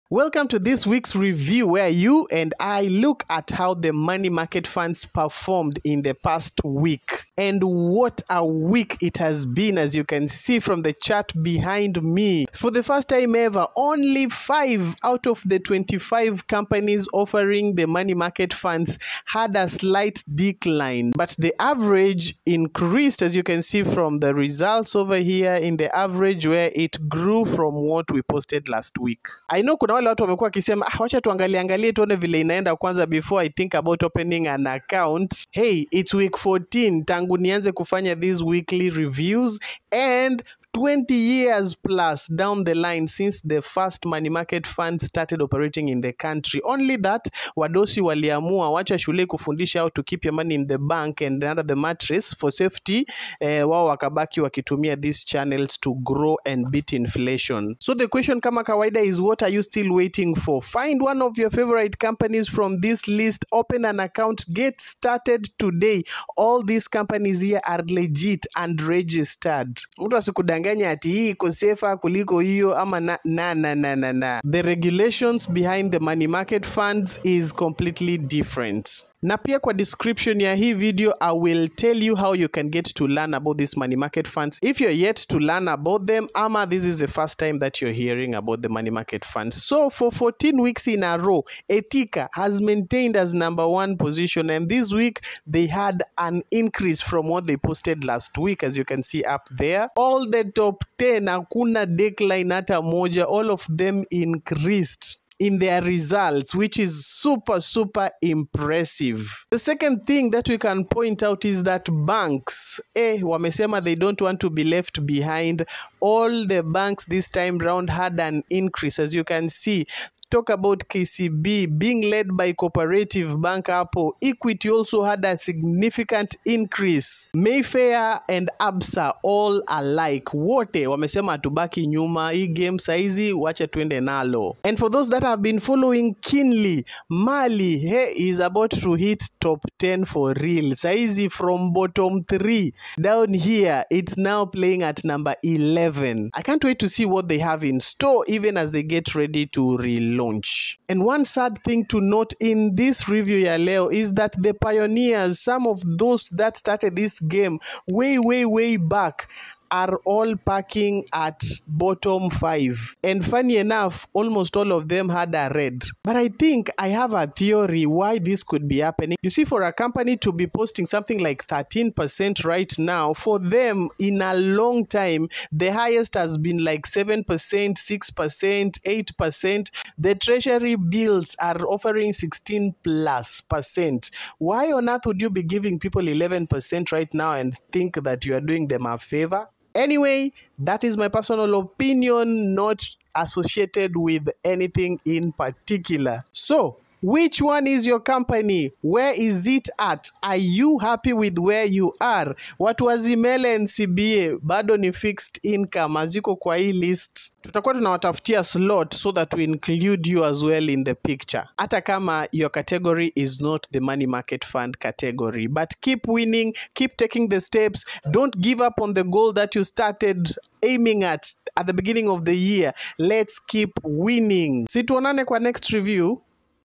Money Market Fund Account Providers Weekly Review in Kenya
This is the money market fund weekly review where we look at the money market fund providers and their results. This is as at the end of March 8th 2024.